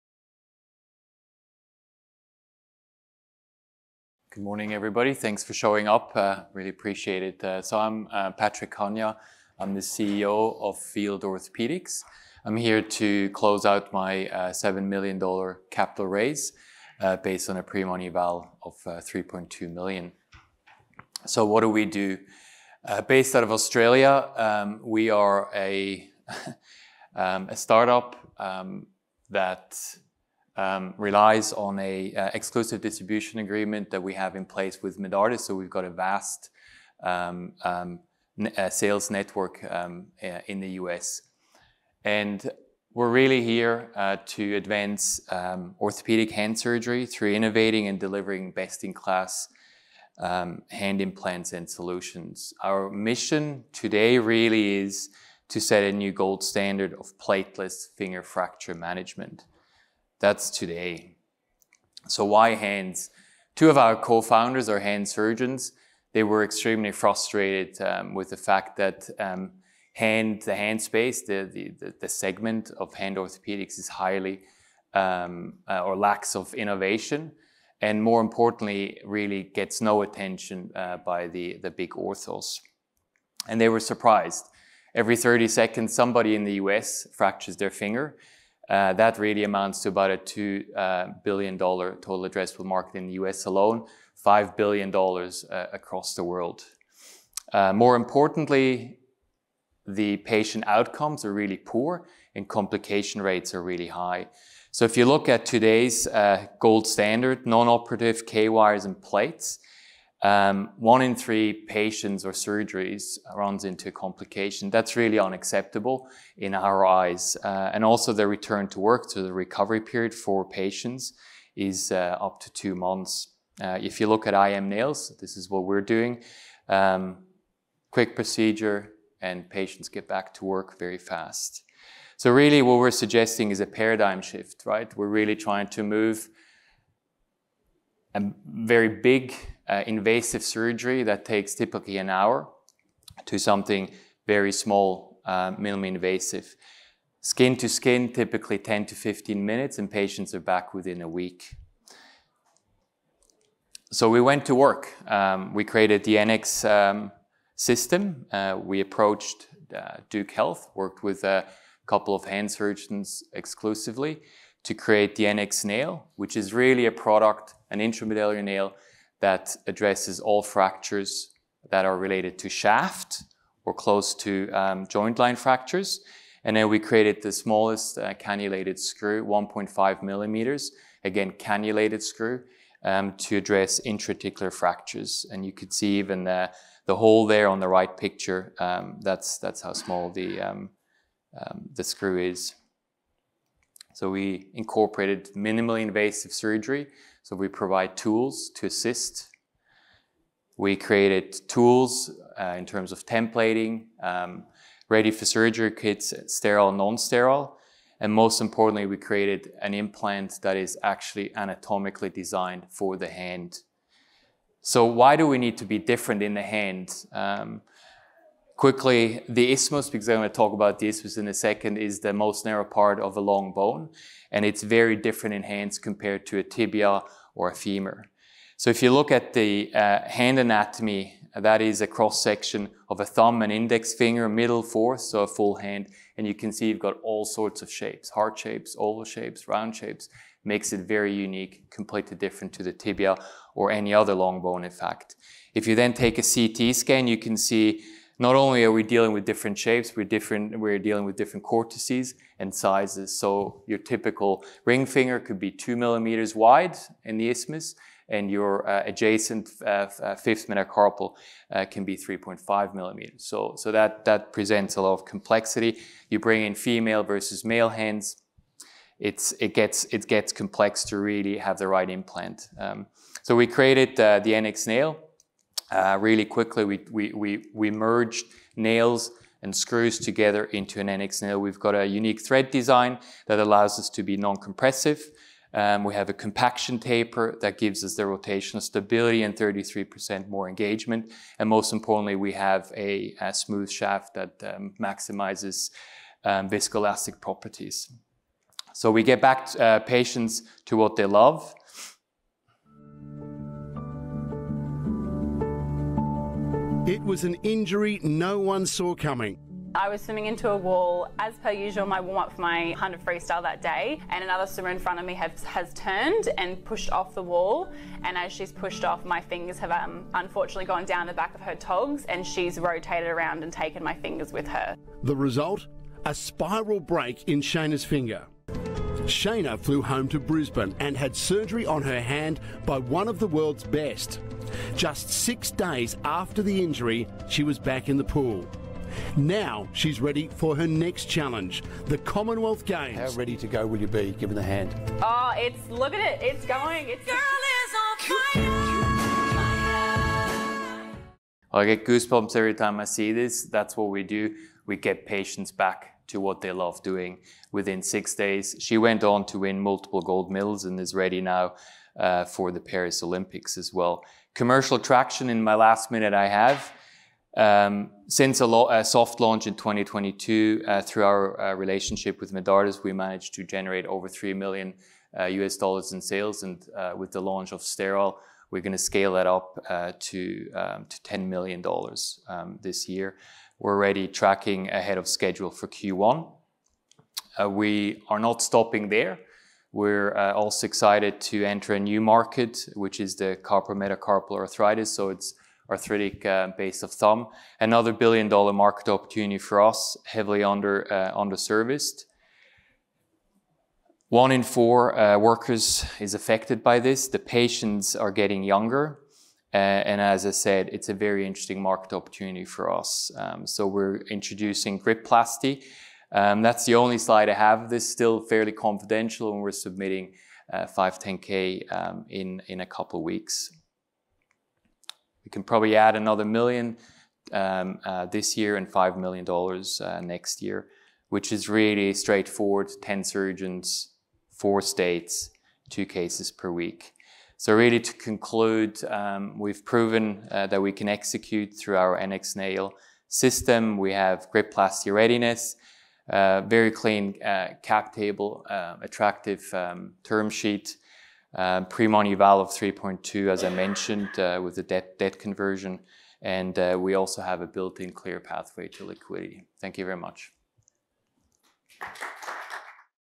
Patryk Kania, Field Orthopaedics - Cannulated Screws & Intramedullary Nailing System | LSI USA '24
Patryk Kania presented Field Orthapaedics at LSI USA ‘24 Emerging Medtech Summit.

Field Orthopaedics is dedicated to developing innovative products to meet the needs of complex fracture management, arthritis and microsurgery.